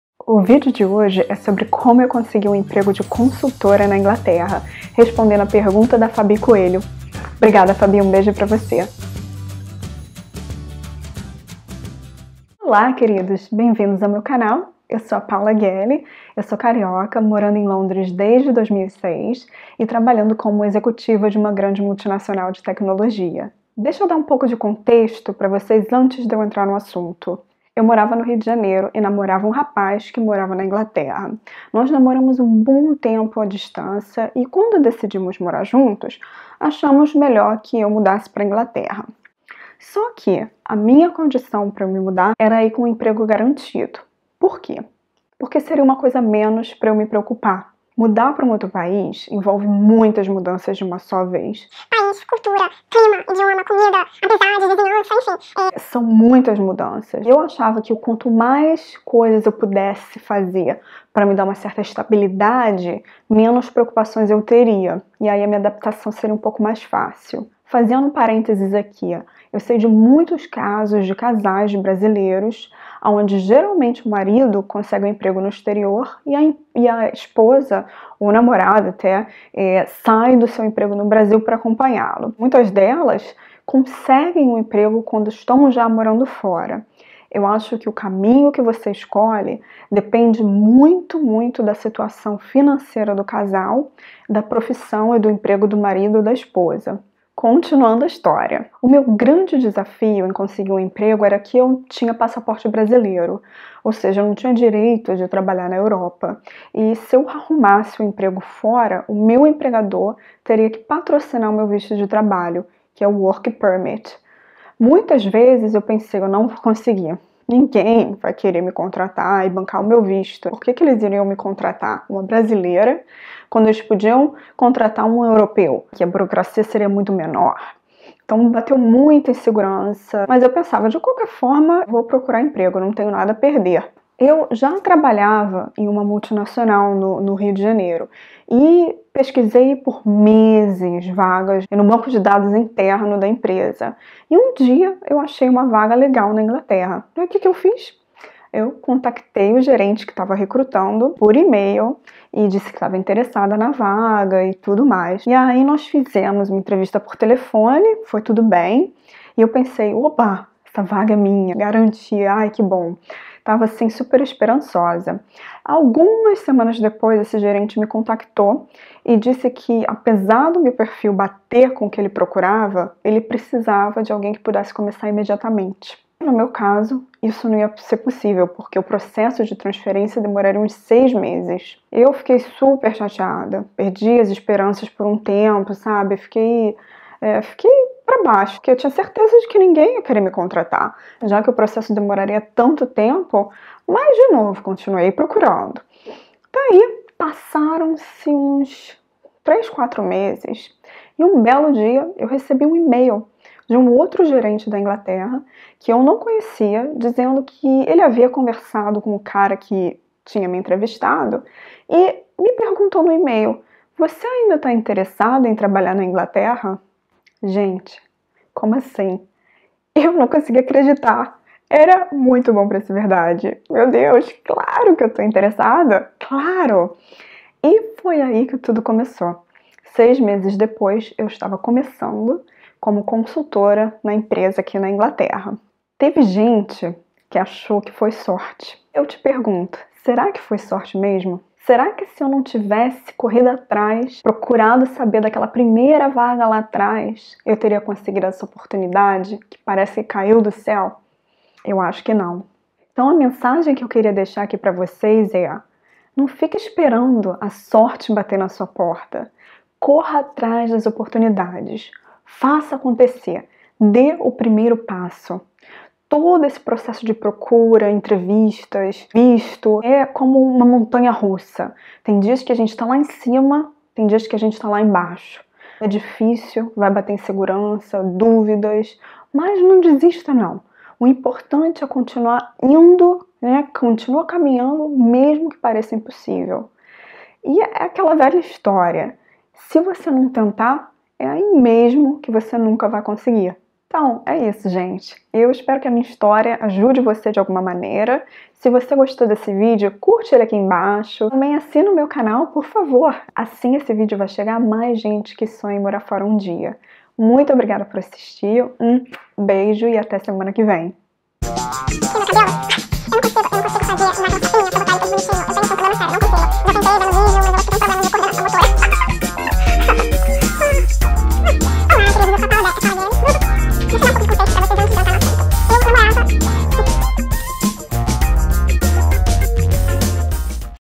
Como eu consegui emprego na Europa
Como consegui emprego de consultora na Europa tendo passaporte brasileiro e sem indicação de ninguém.